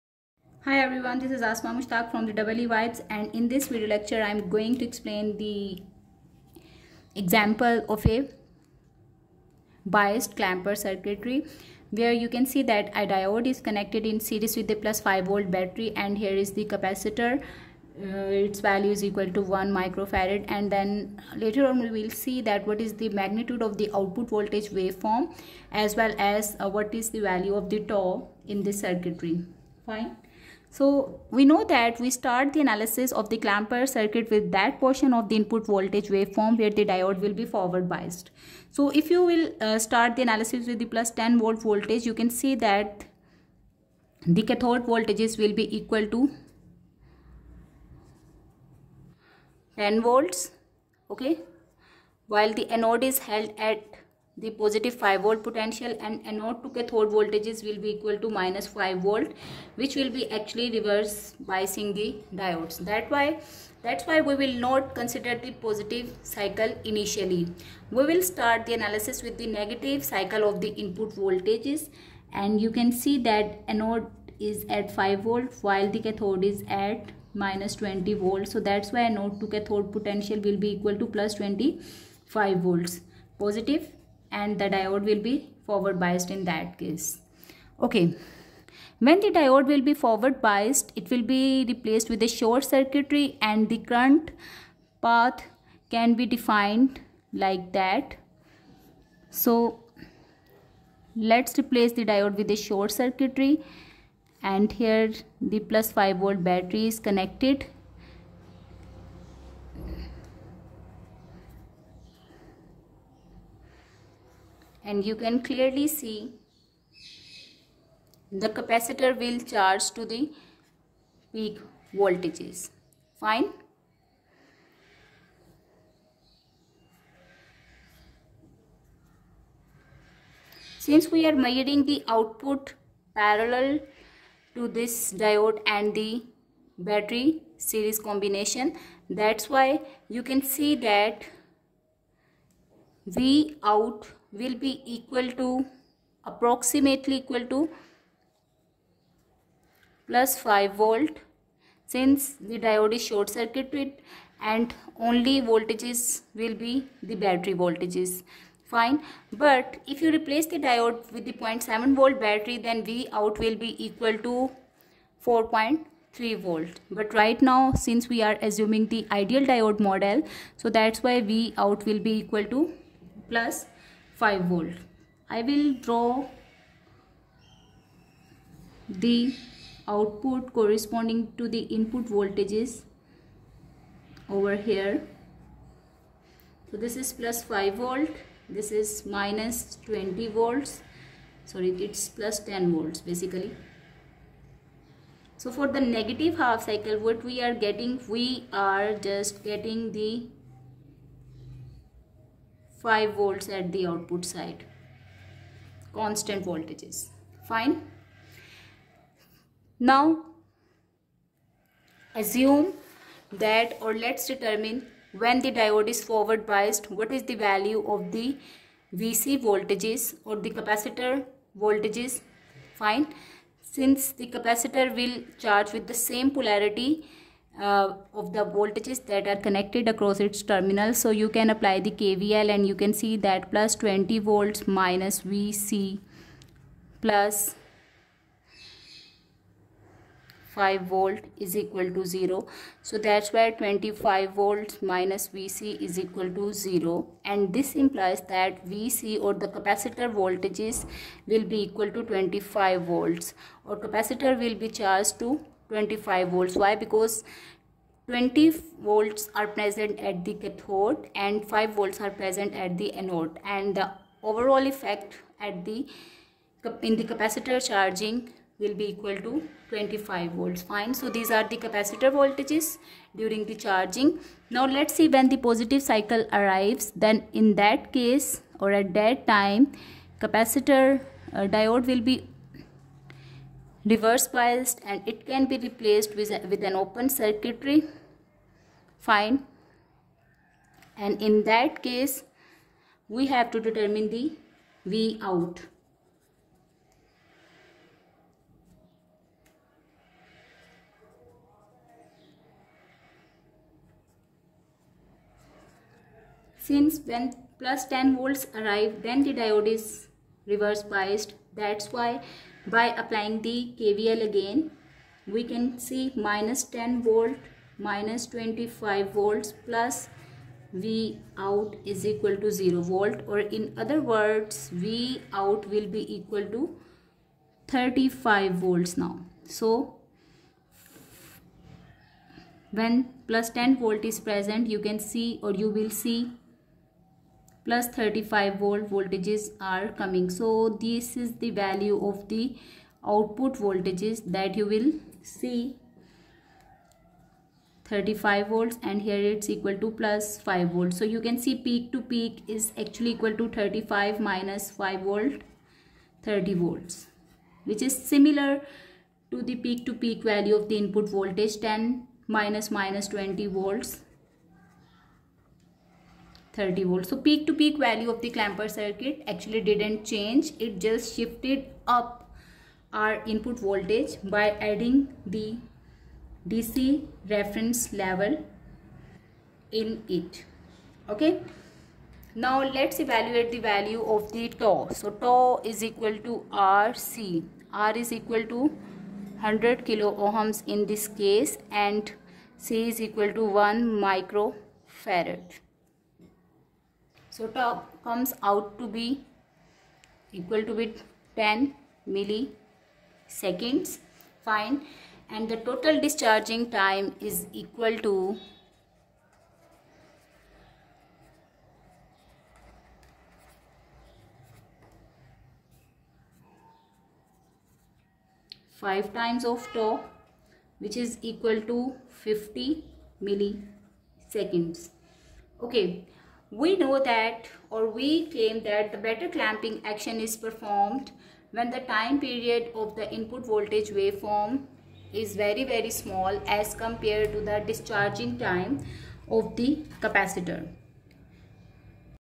Example of Biased Clamper Circuits
Clampers are also known as the DC level restorers. Clampers are used for adding a positive or negative DC voltage levels to the input voltage waveform. Clampers do not change the shape of input voltages as well as the peak-to-peak voltages of input waveform.

Clamper circuits are electronic circuits used to shift the DC level of a waveform without altering its shape. They are commonly used to establish a reference voltage or to "clamp" the waveform to a desired DC level. Clamper circuits typically consist of a diode, a capacitor, and a resistor.

The diode in a clamper circuit allows current to flow in only one direction, either charging or discharging the capacitor depending on the polarity of the input waveform. The capacitor stores charge and helps to establish the desired DC level. The resistor is used to control the charging or discharging time of the capacitor.